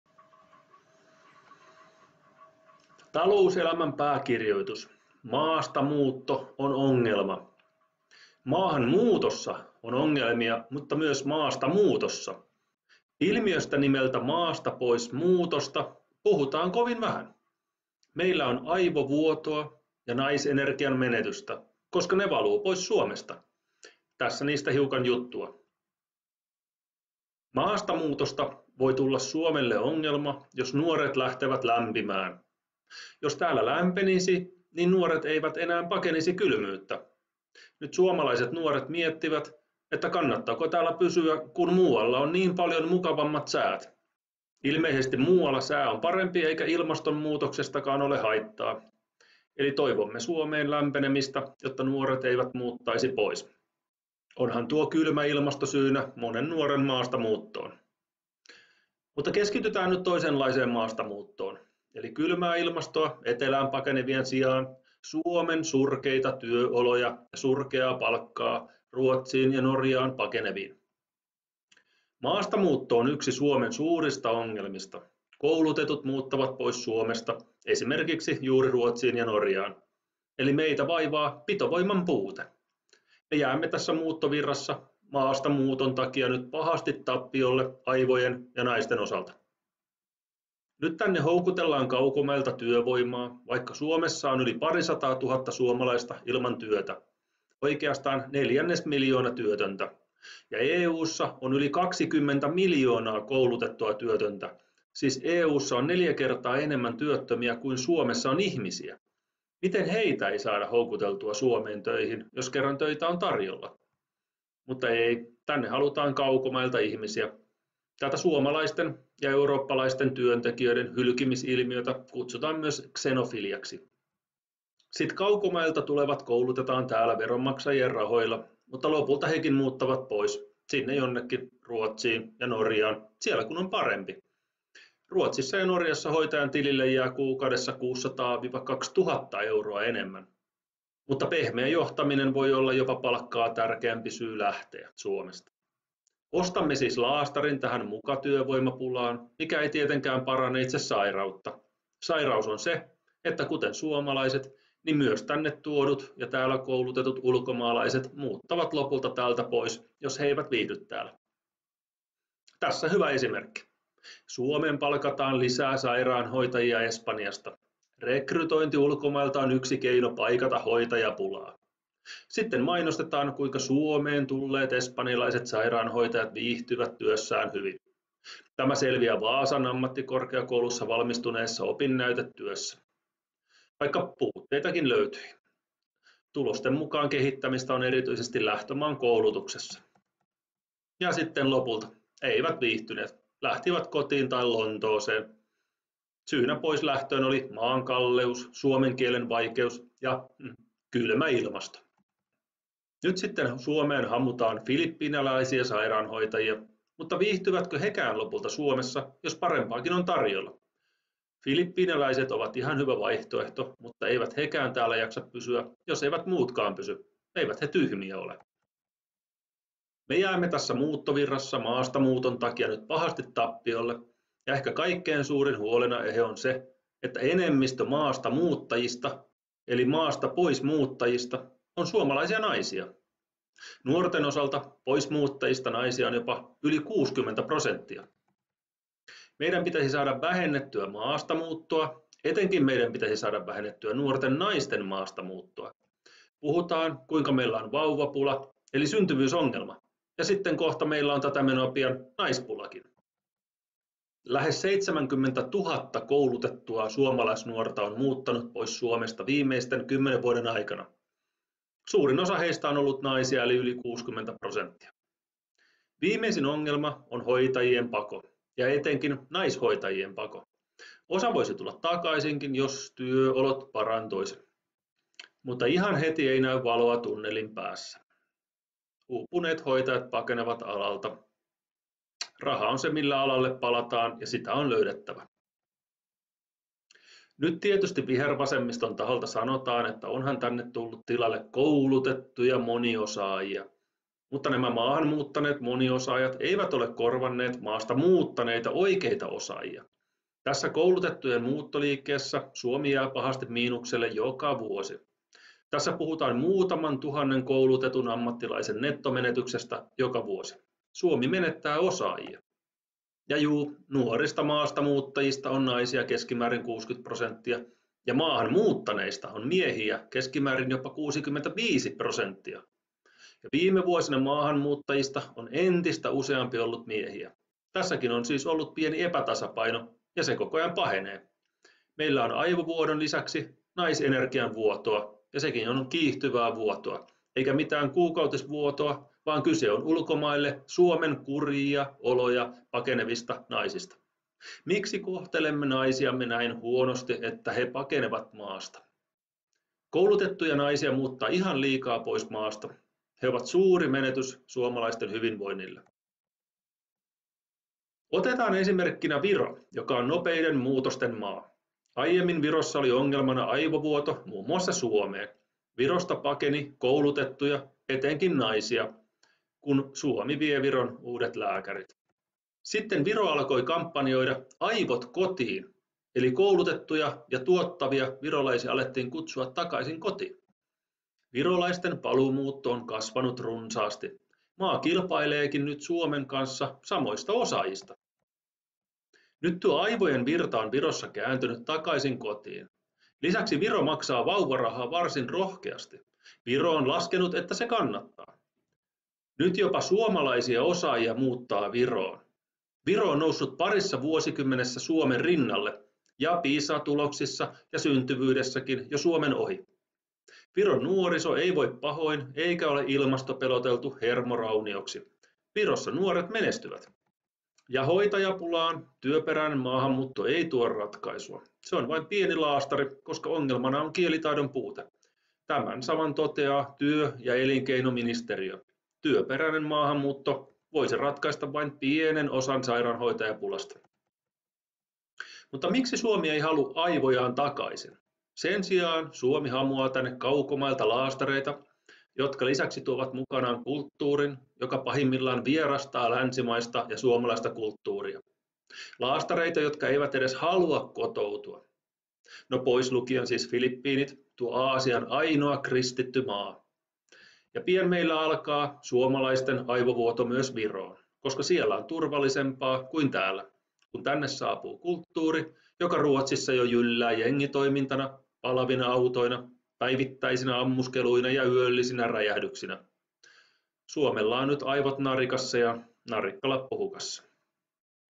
Naiset pakenevat Suomesta!
Nuoret naiset pakenevat Suomesta kurjia työoloja!
Suomessa on iso ongelma:
- koulutettujen naisten joukkopako
= eli koulutettujen naisten maastamuutto!
= aivovuotoa kurjien työolojen takia!
Ja tilalle tulee kouluttamattomia miehiä.
Tuontilaastari vai korjausliike - mitä pitäisi tehdä?